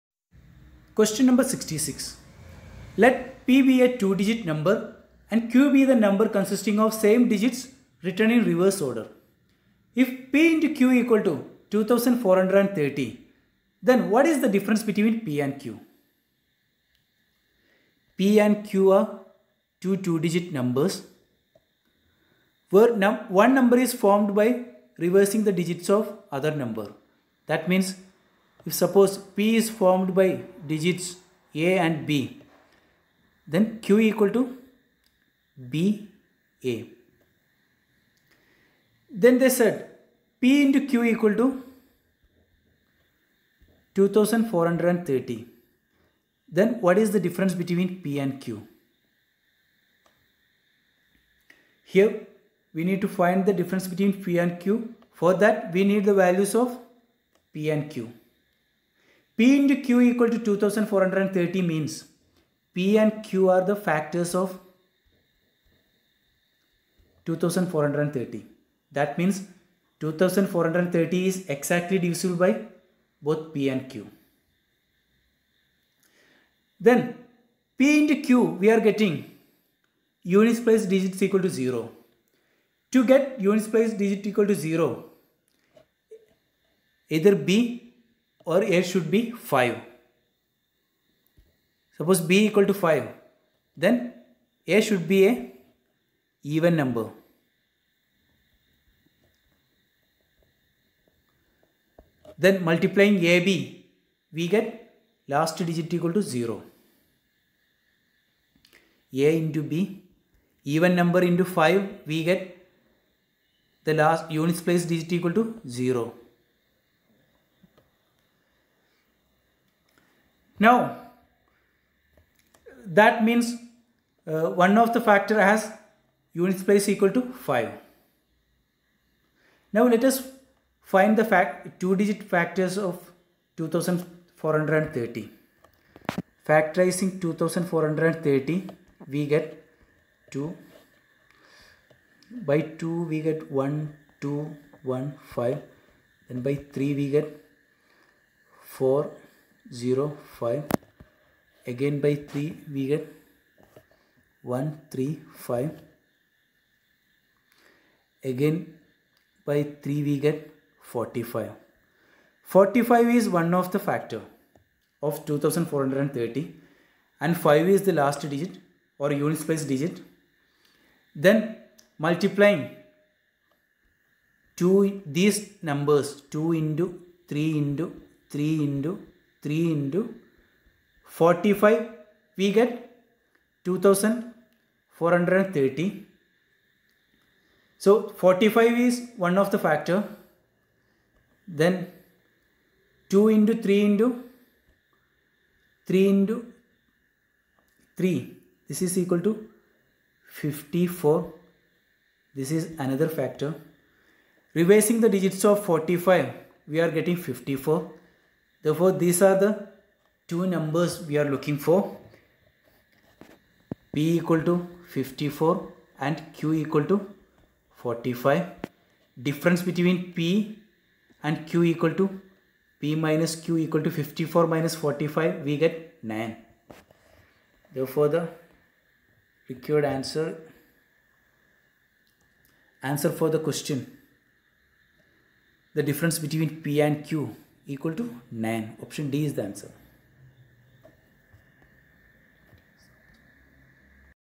2022_IAS_Qn A66. Let p be a two-digit number and q be the number consisting of same digits
Let p be a two-digit number and q be the number consisting of same digits written in reverse order. If p × q=2340, then what is the difference between p and q?

(a)    45
(b)    27
(d)    9